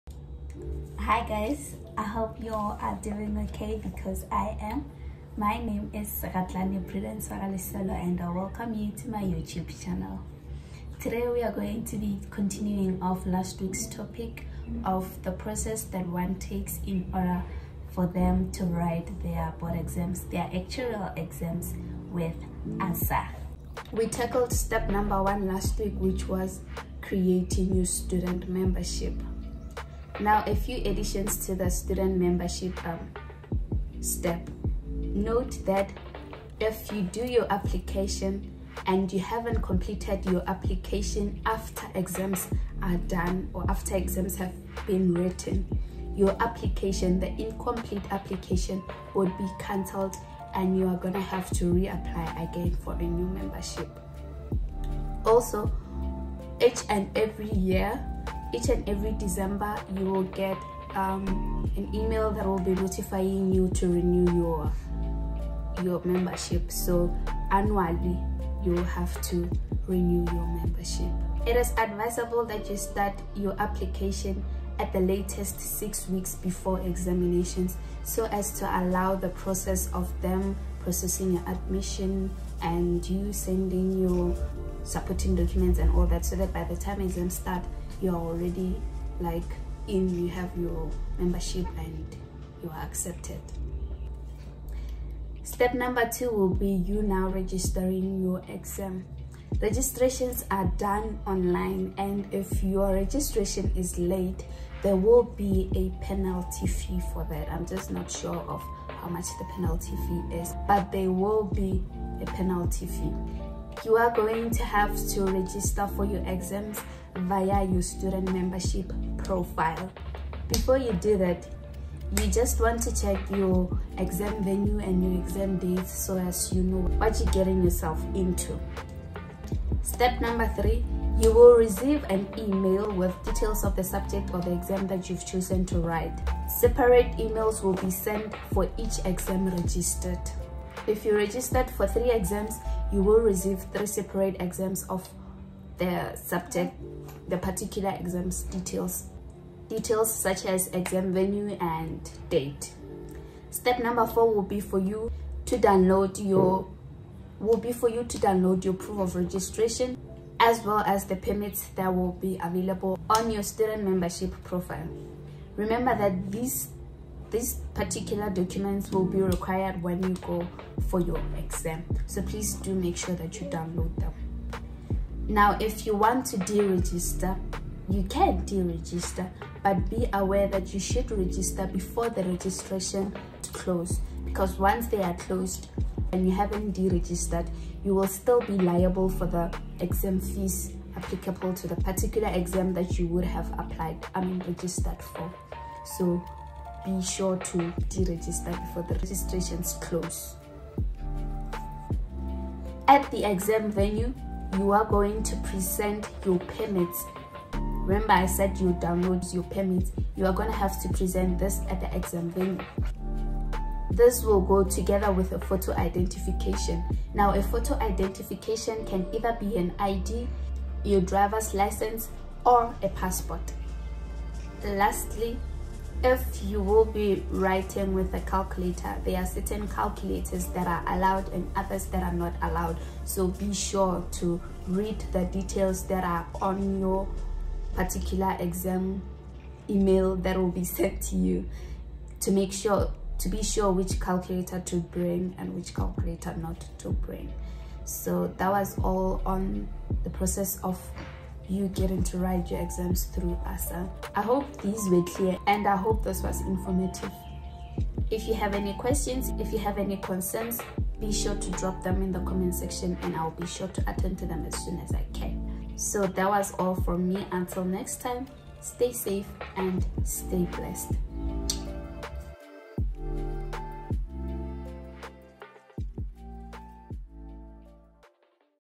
Writing Actuarial exams || ASSA || Registration
Process on how to get to writing your Actuarial sciences exams through ASSA

POSTAL: Actuarial Society of South Africa
                       Cape Town
                        8000

COURIER: Actuarial Society of South Africa
                  c/o Old Mutual Office 2
                  2nd Floor West End Mall
                 Jan Smuts Drive
                 Pinelands, 7405

N.B. With the non-accredited universities, in addition to the two referees recommendation forms, you will need to add a certified colour copy not older than 6-months of your degree obtained.